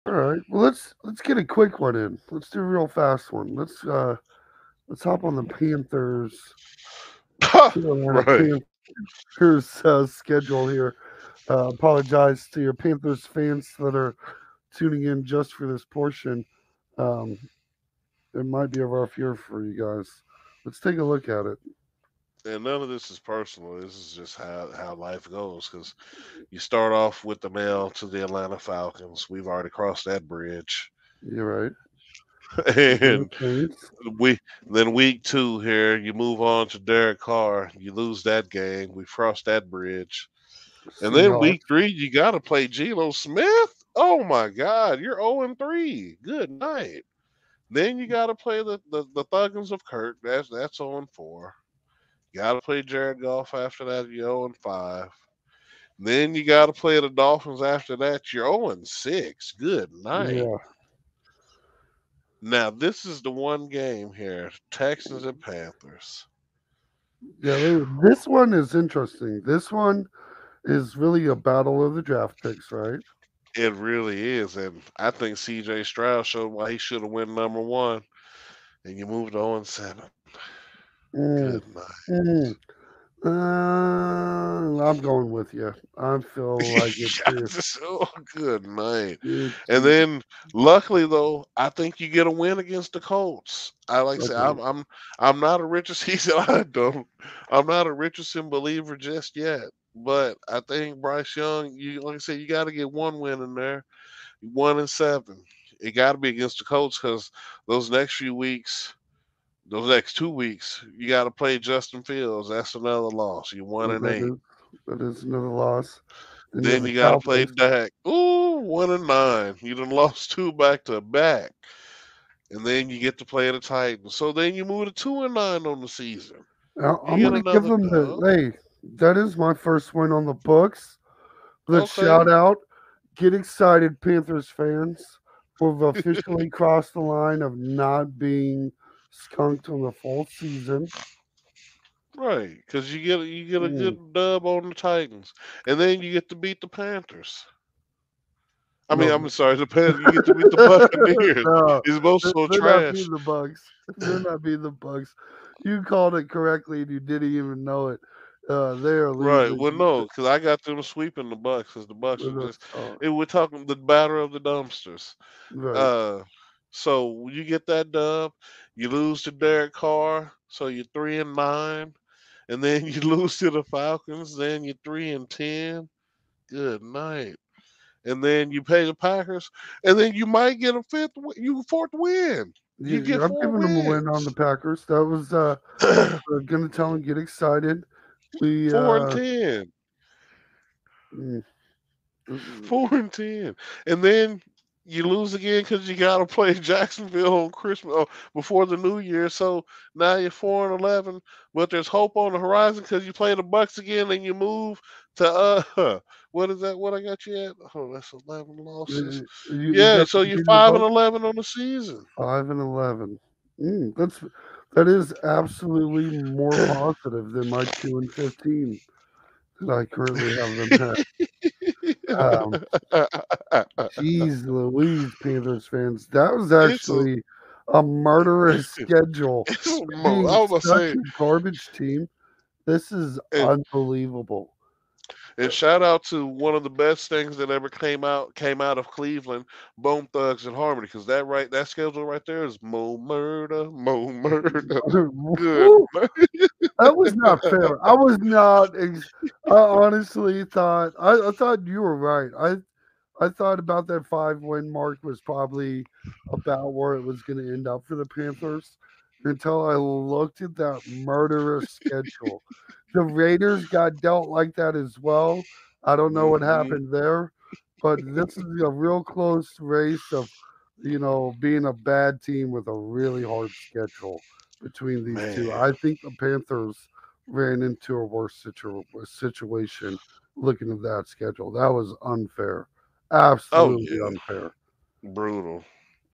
Carolina Panthers 2023-24 Season Predictions | Full Schedule Breakdown
your host Mr KC from the 813 & that dude with a beard bring you Carolina Panthers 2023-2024 predictions. this video is from our live podcast where  we do predictions for the entire NFC South
including

Atlanta Falcons 2023 predictions
New Orleans Saints 2023 predictions
Tampa Bay Buccaneers 2023 predictions
Carolina Panthers  2023 Predictions